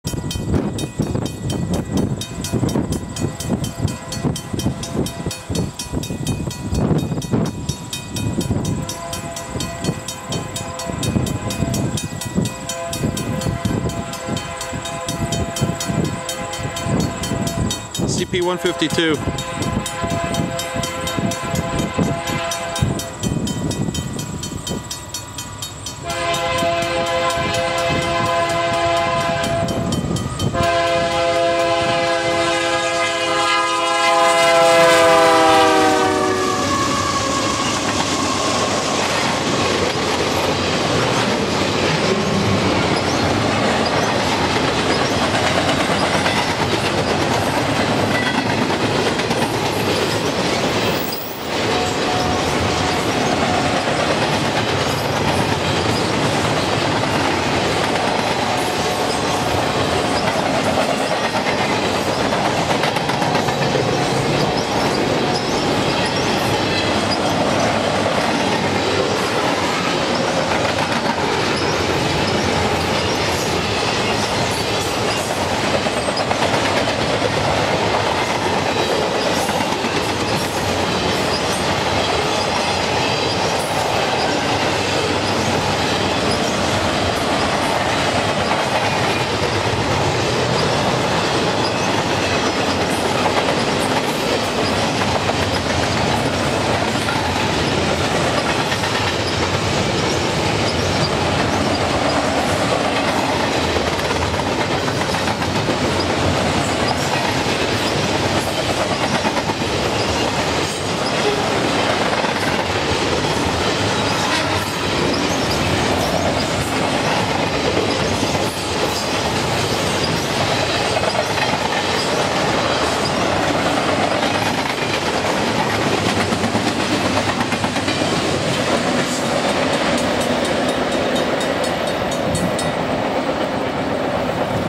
CP 152 east thru Tilbury. Sept 11 2010
CP 152 led by 9553 & 9648 (AC4400CW) hauls this long intermodal train eastbound thru Tilbury. Video shot at 12:16pm